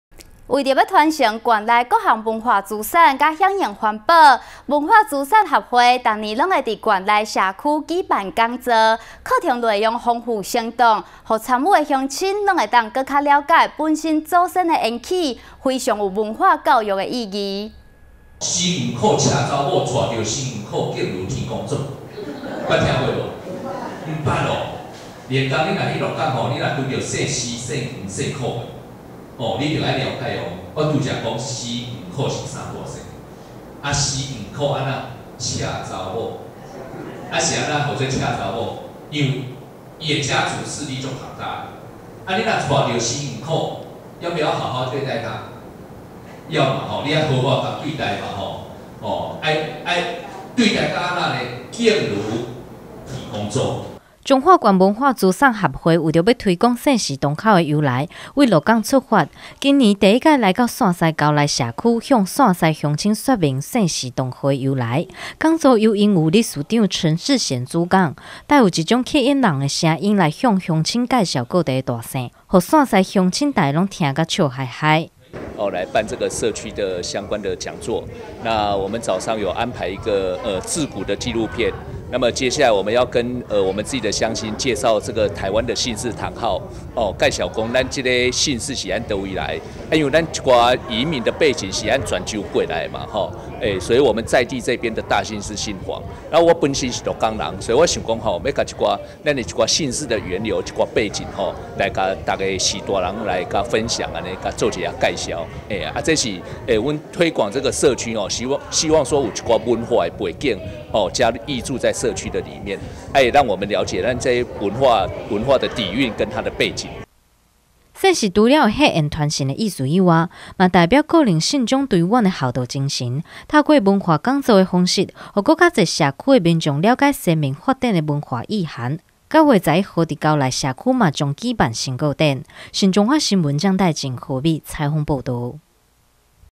新彰化新聞0817 傳承姓氏堂號由來 文化資產學會巡迴社區
為了傳承縣內各項文化資產及提倡環保，文化資產學會每年都會在縣內社區舉辦講座；課程內容豐富生動，讓參與的鄉親都可以更加瞭解本身祖先的緣起，非常具有文化教育意義。